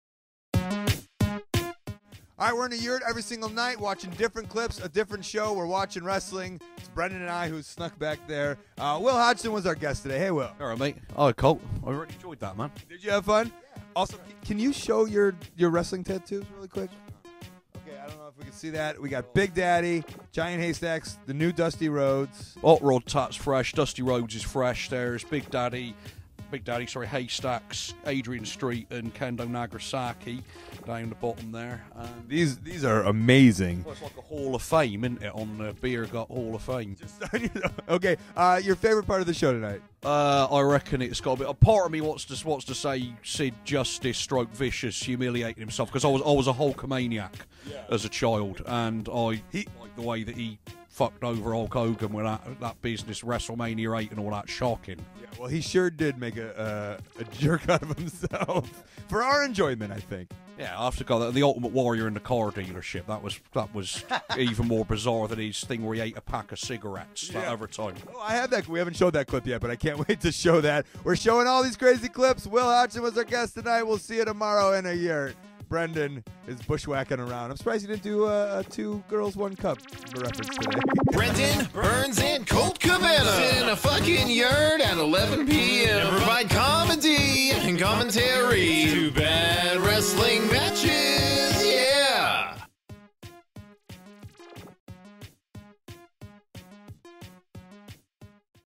2015 Ed Fringe RECAP Show #5 Colt Cabana & Brendon Burns w/ Wil Hodgson
SHOW #5 - Tuesday  Aug 11th
THE LIVE SHOW:
Brendon Burns & Colt Cabana sit in a F*cking Yurt at 11pm and do Comedy & Commentary to BAD WRESTLING MATCHES

OUR GUEST:
WIl Hodgson
HIS LIVE SHOWS:
6:20pm "Wil Hodgson- Paranoid"